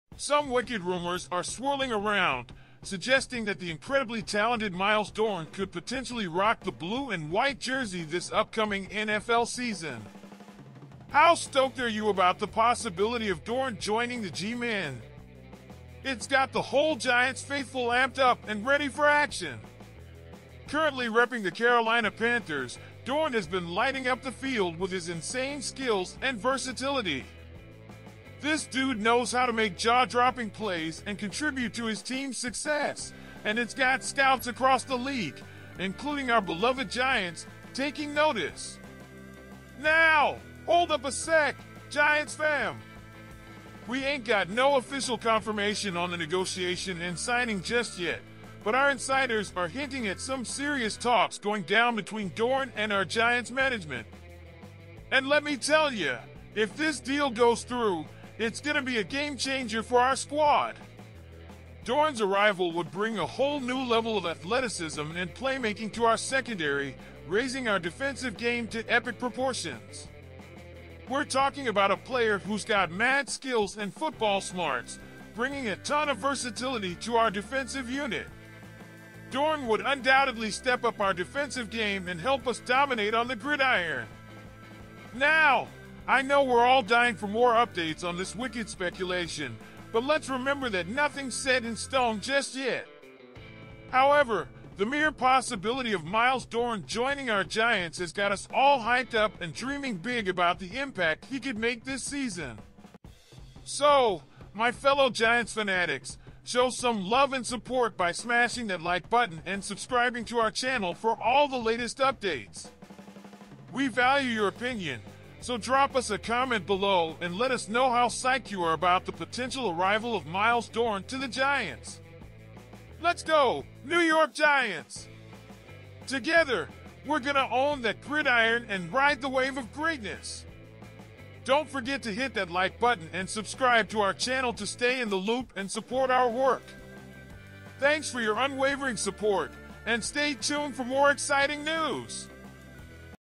💣 BIG NEWS! LOOK WHAT WAS ON TV! NEW YORK GIANTS NEWS TODAY!
Big Blue Nation Buzzing as Myles Dorn May Be Big Blue Bound for Upcoming NFL Season

New York, USA - Yo, Giants fans! Some wicked rumors are swirling around, suggesting that the incredibly talented Myles Dorn could potentially rock the blue and white jersey this upcoming NFL season! How stoked are you about the possibility of Dorn joining the G-Men? It's got the whole Giants faithful amped up and ready for action!

Currently repping the Carolina Panthers, Dorn has been lighting up the field with his insane skills and versatility. This dude knows how to make jaw-dropping plays and contribute to his team's success, and it's got scouts across the league, including our beloved Giants, taking notice.

Now, hold up a sec, Giants fam. We ain't got no official confirmation on the negotiation and signing just yet, but our insiders are hinting at some serious talks going down between Dorn and our Giants management. And let me tell ya, if this deal goes through, it's gonna be a game-changer for our squad. Dorn's arrival would bring a whole new level of athleticism and playmaking to our secondary, raising our defensive game to epic proportions.

We're talking about a player who's got mad skills and football smarts, bringing a ton of versatility to our defensive unit. Dorn would undoubtedly step up our defensive game and help us dominate on the gridiron.

Now, I know we're all dying for more updates on this wicked speculation, but let's remember that nothing's set in stone just yet. However, the mere possibility of Myles Dorn joining our Giants has got us all hyped up and dreaming big about the impact he could make this season.

Let's go, New York Giants! Together, we're gonna own that gridiron and ride the wave of greatness!